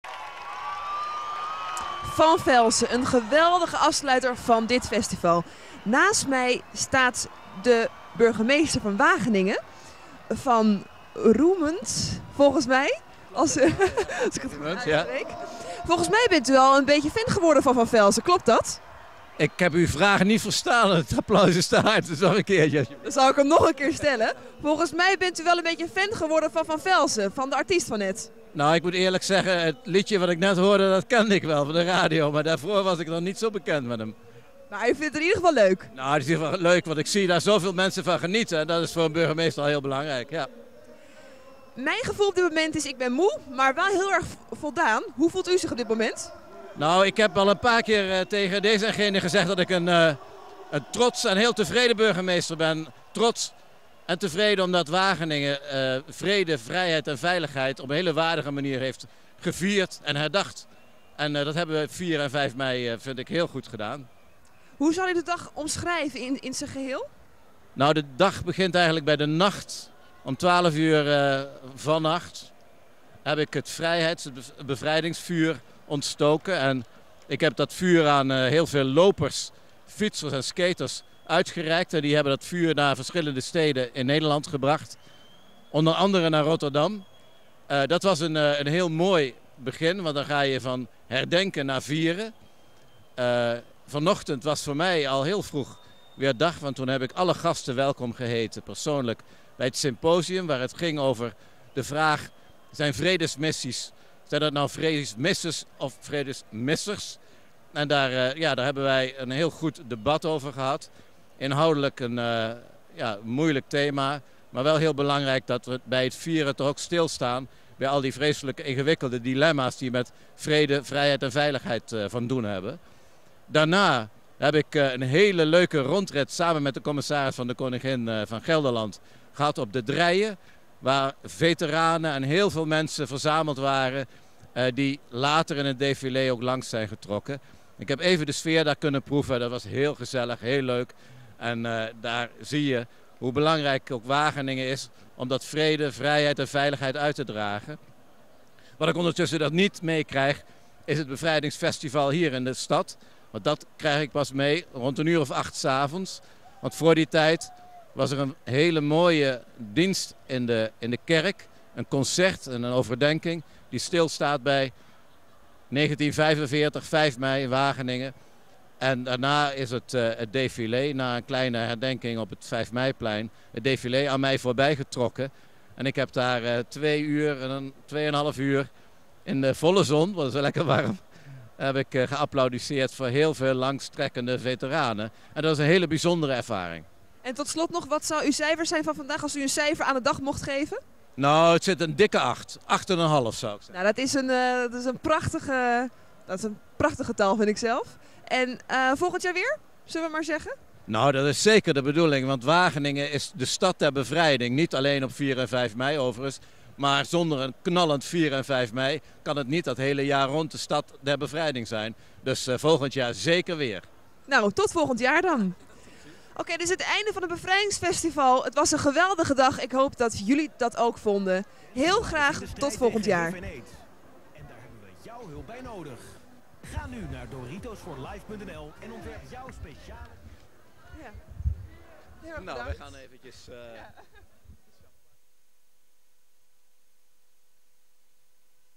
Geert van Rumund - Interview @Bevrijdingsdag 2008 Wageningen
Een interview met Burgemeester Geert van Rumund na afloop van het optreden van VanVelzen op het Bevrijdingsfestival te Wageningen, 5 mei 2008.